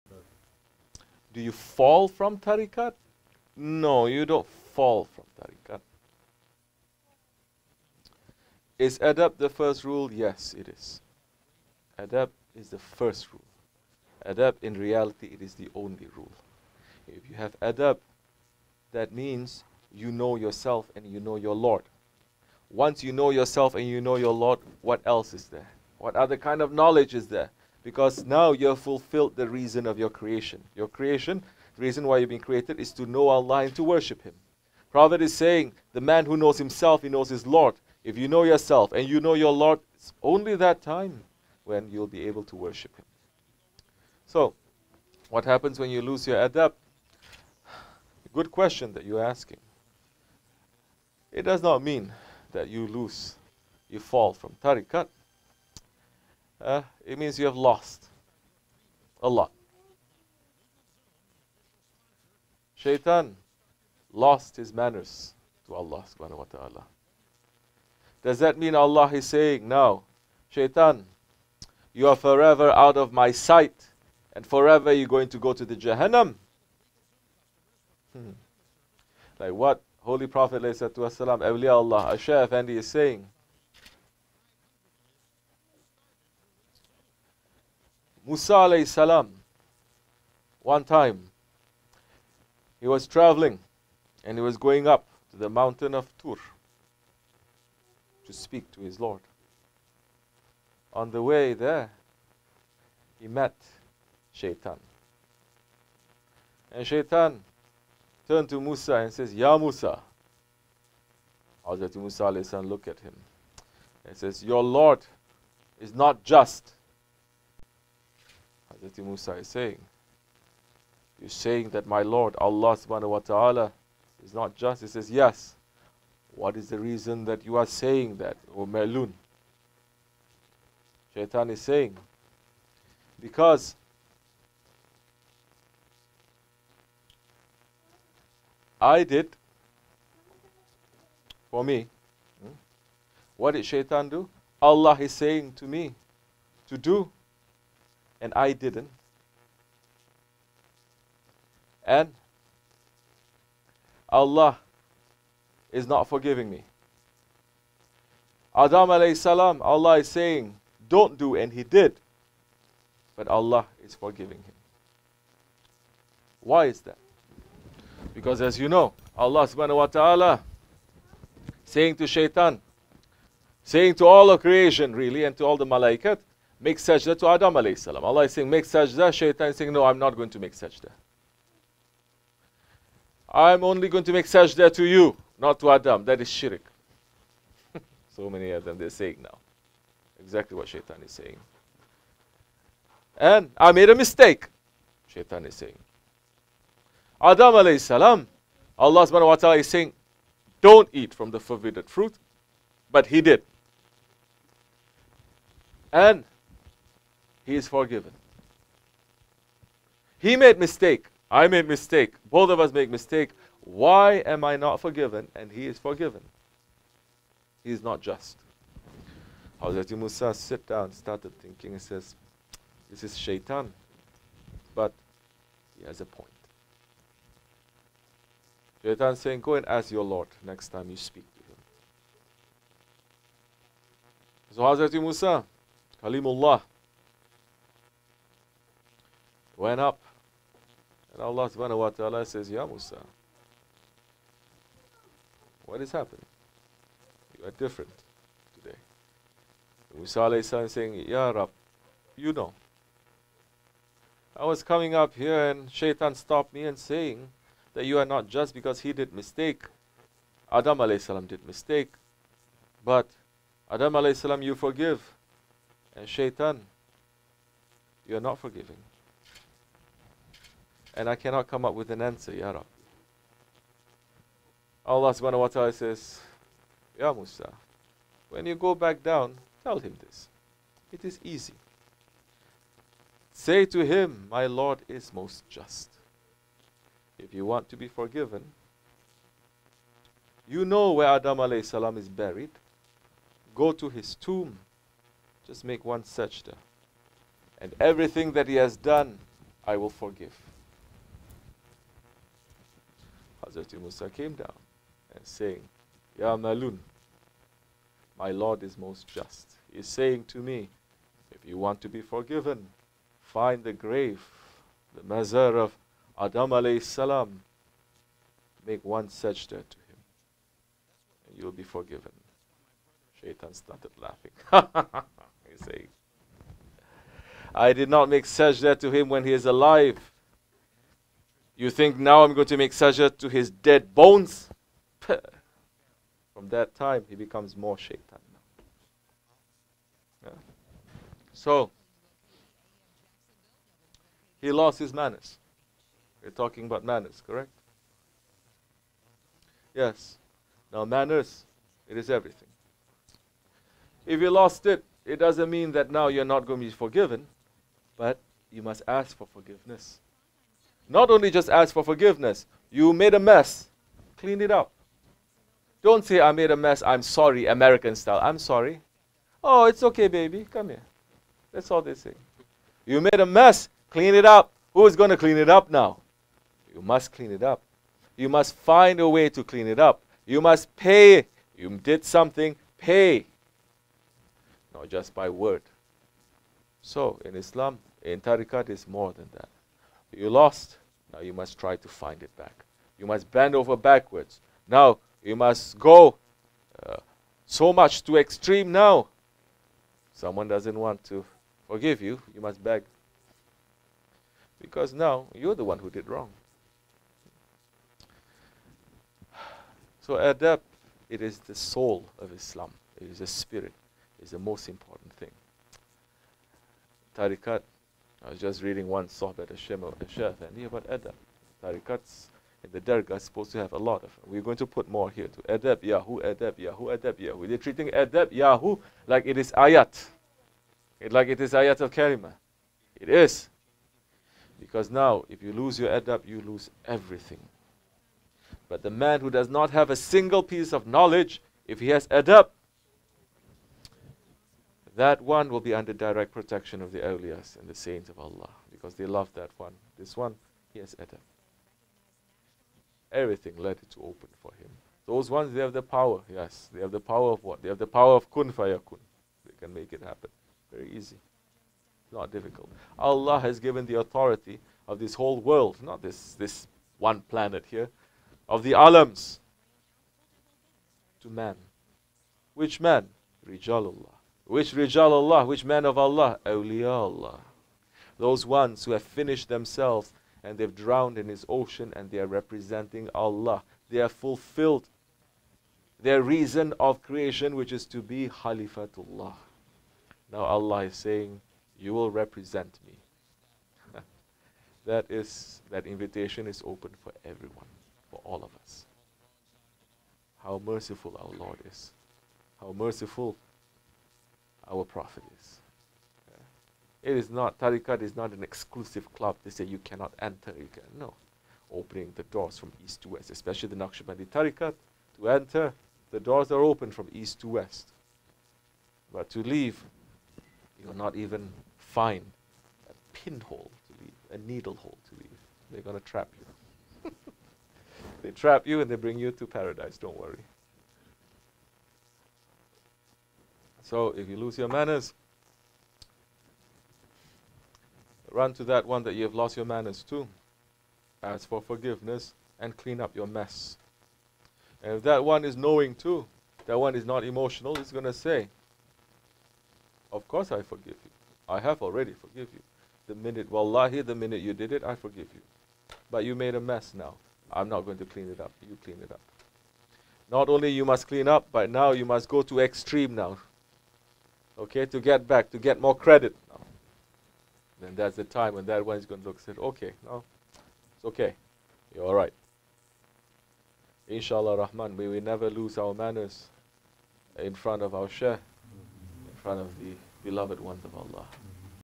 Q&A: "Does one who loses adab fall from the Tariqa?"
* QUESTION: "Is adab (good manners) the first rule that we must learn in Tarikat? If we lose adab, do we fall from the Tarikat?"
Live Question & Answer session with Lokman Efendi, the khalifah of Shaykh Abdulkerim el Kibris (qs)
Osmanli Dergahi - Florida